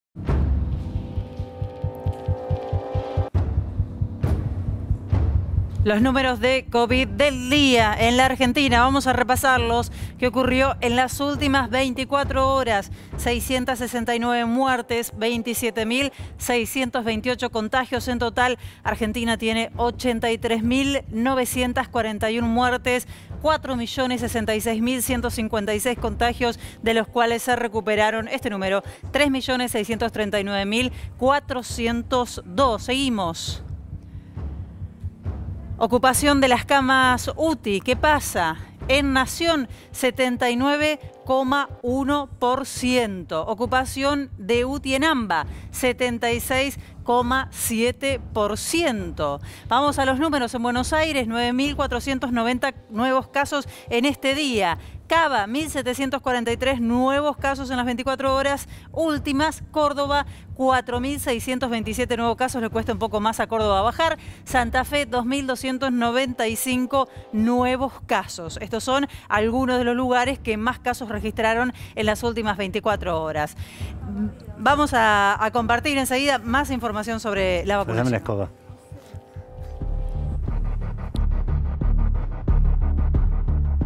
Coronavirus en la Argentina: último parte
Otras 669 personas murieron y 27.628 fueron reportadas con coronavirus en las últimas 24 horas en el país, con lo que suman 83.941 los fallecidos registrados oficialmente a nivel nacional y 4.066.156 los contagiados desde el inicio de la pandemia, informó este jueves el Ministerio de Salud de la Nación. La cartera sanitaria indicó que son 7.817 los internados con coronavirus en unidades de terapia intensiva, con un porcentaje de ocupación de camas de adultos de 79,1% en el país y del 76,6% en la Área Metropolitana Buenos Aires.